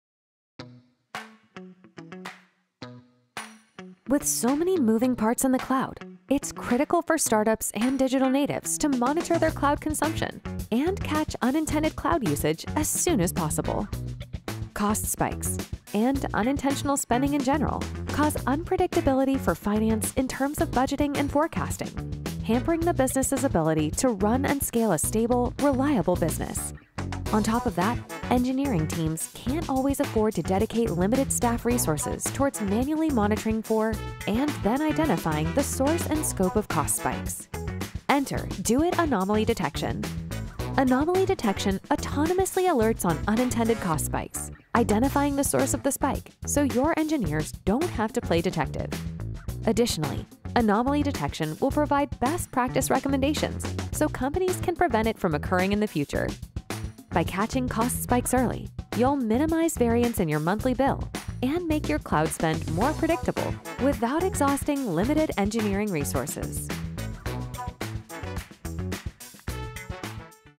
DoiT Anomaly Detection Overview
With so many moving parts in the cloud, it’s critical for you to monitor cloud consumption and catch unintended cloud usage as soon as possible.

Otherwise you'll have to deal with unpredictability in terms of budgeting and forecasting, hampering your ability to run and scale a stable, reliable business.

However, engineering teams can’t always afford to dedicate limited staff resources towards manually monitoring costs or build their own automated monitoring.

Enter DoiT Anomaly Detection, which autonomously alerts on unintended cost spikes, identifying the source(s) of the spike and offering best practice recommendations so you can prevent it from occurring in the future.